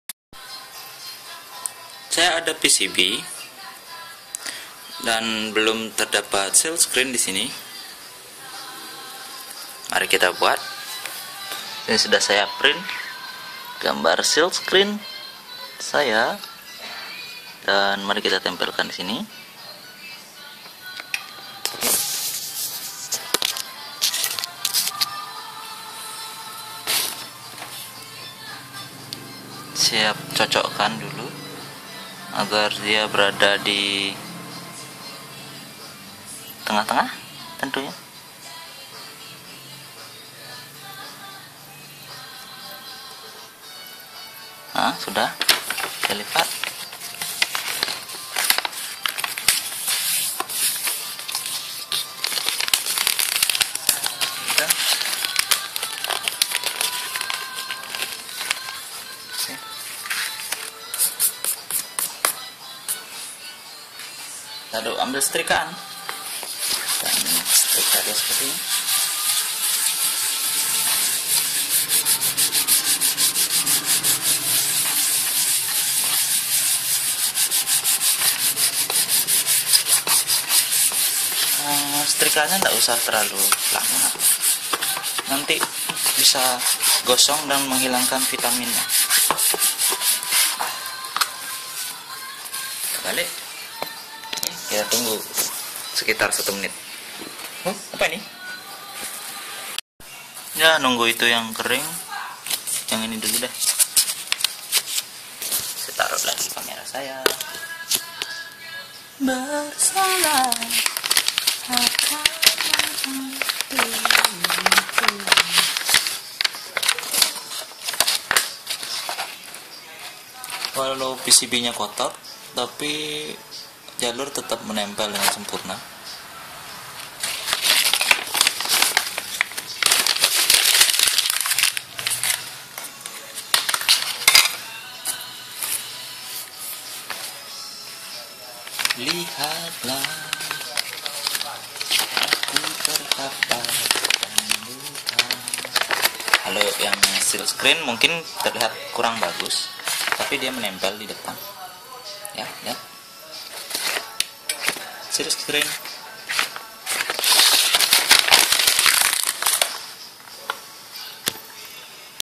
Membuat SIlk Screen PCB dengan Mudah
Buat layout nya gampang, sampai bikin silk screen nya pun gampang juga :D